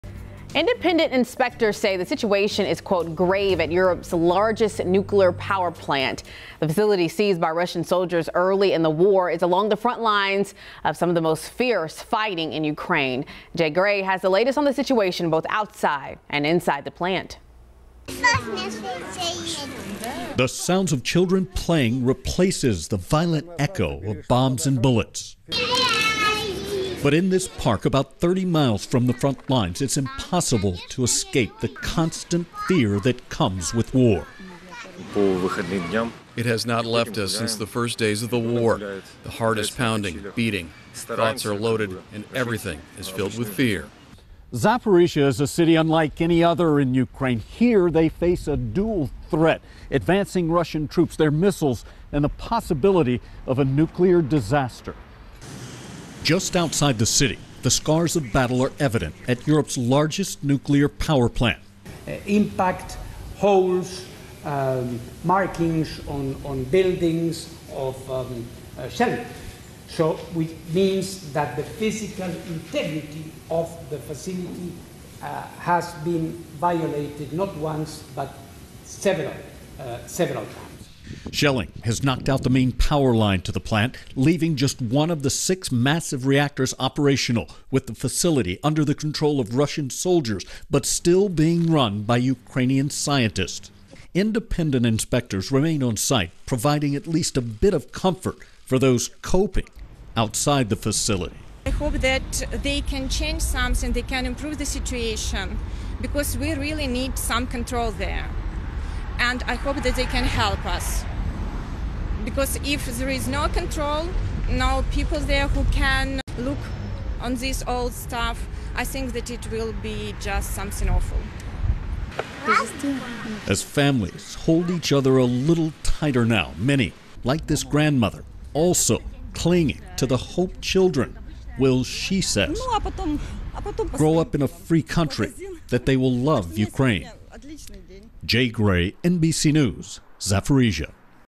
Nuclear fears with Ukranian power plant
The head of the U.N. nuclear watchdog said Saturday that the Russian-controlled Zaporizhzhia nuclear plant in Ukraine was disconnected to its last external power line but was still able to run electricity through a reserve line amid sustained shelling in the area.

International Atomic Energy Agency Director-General Rafael Grossi said in a statement that the agency's experts, who arrived at Zaporizhzhia on Thursday, were told by senior Ukrainian staff that the fourth and last operational line was down. The three others were lost earlier during the conflict.